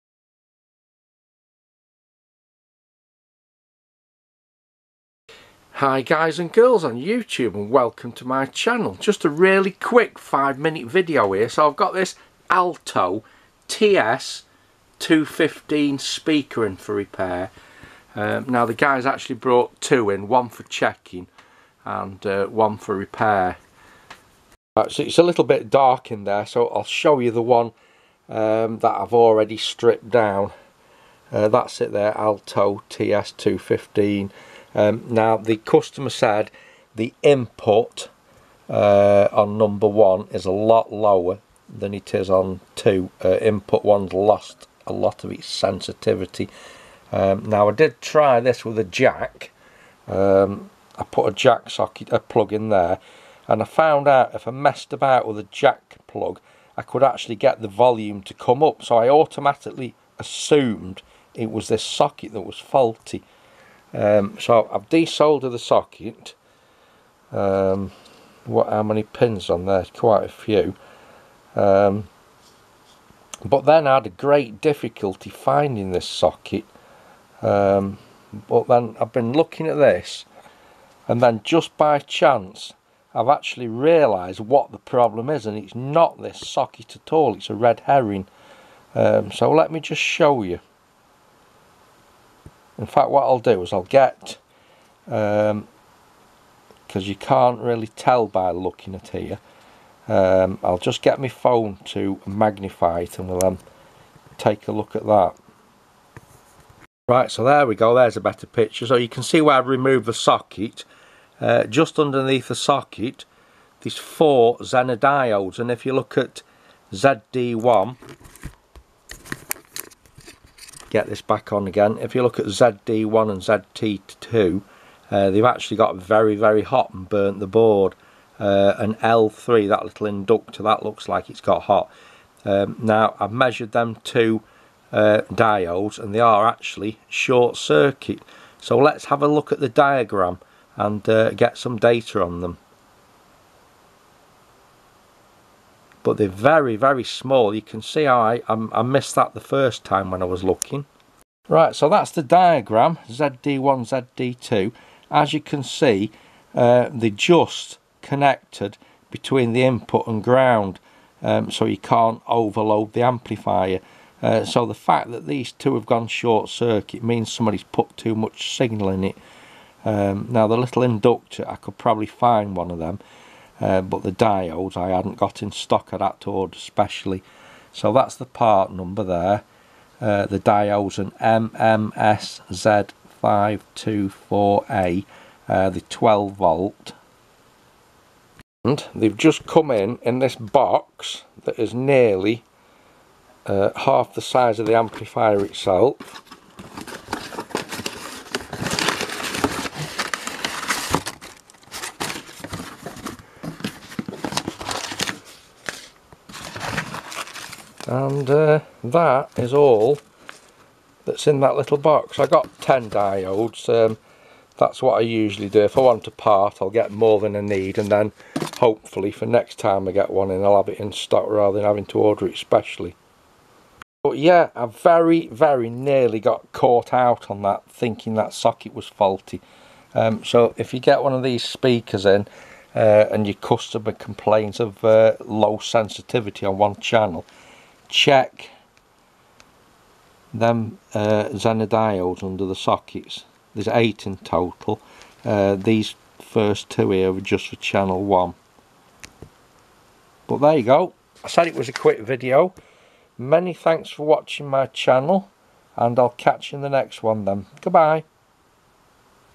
Alto TS215 amplified speaker repair, no sound on CH1
Have a look at the diagram and ask yourself how can these parts fail ???

I nearly made a big  mistake though in assuming the input socket itself was faulty and this is because I was using a mono jack plug so the sound came back on  as you pulled out the jack, the circuit diagram reveals all.

So how did they fail in the first place , well the customer had the answer to this when he came to collect the speaker, he connected the output of a audio power amplifier to the line input on the Alto.